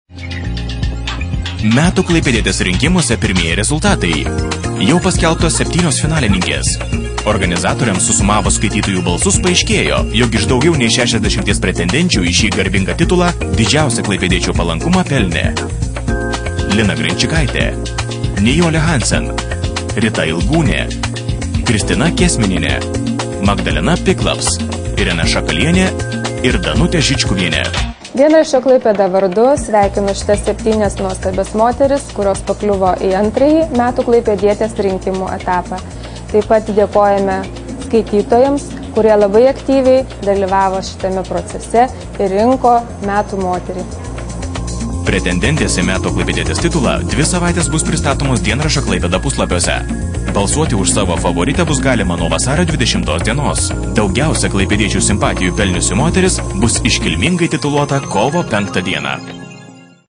METŲ KLAIPĖDIETĖ - išrinktas moterų septynetukas
Balsuokite ir išrinkite moterį, kurią galima būtų tituluoti aštuntąja Metų klaipėdiete.